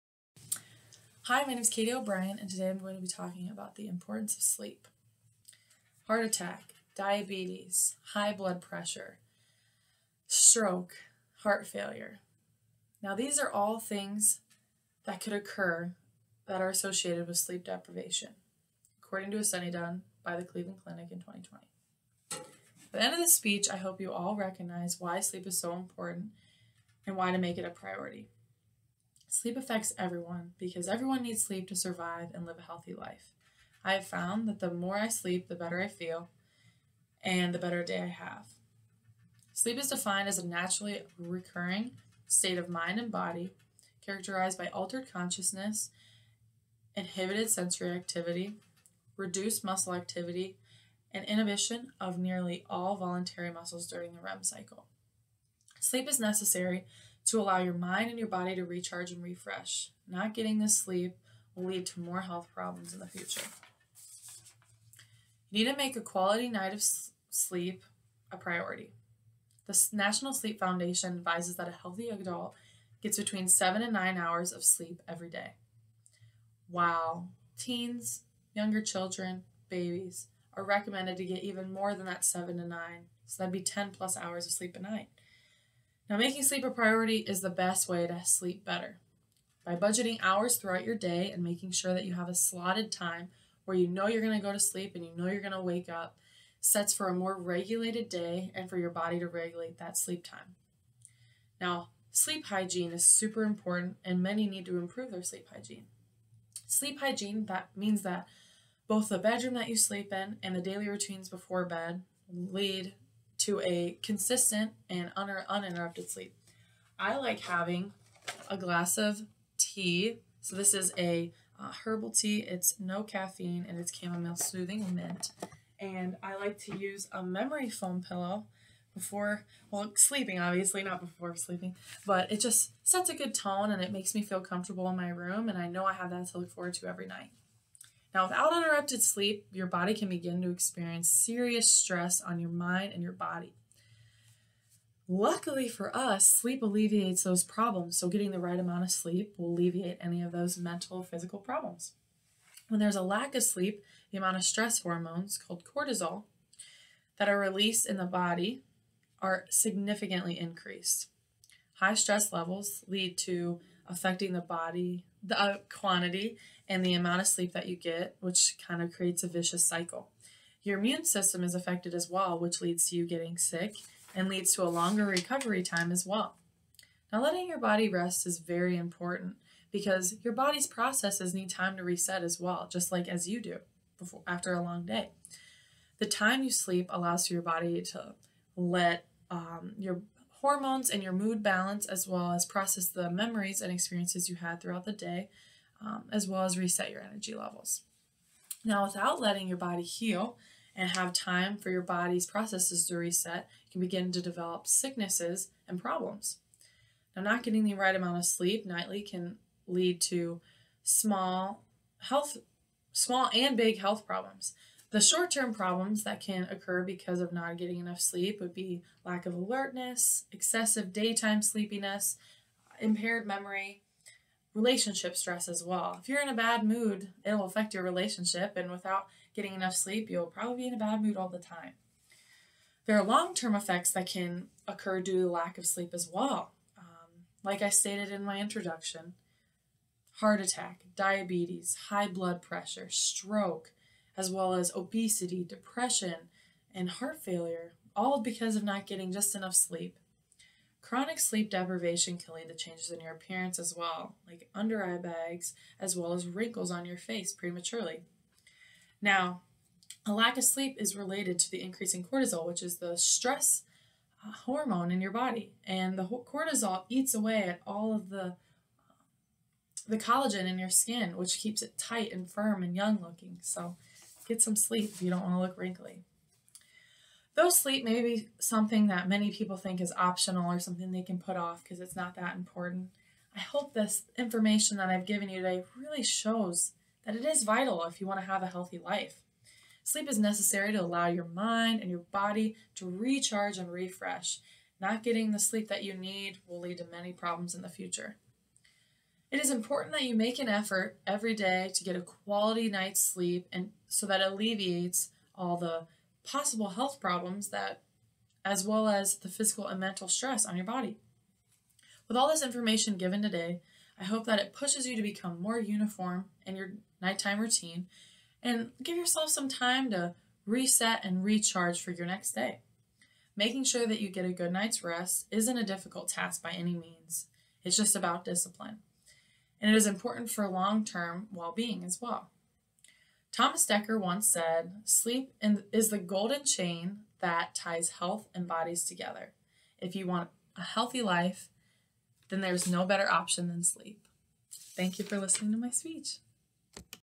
The importance of sleep- Katie O'brien
Sleep is vital to your everyday life!! Learn why it is here :)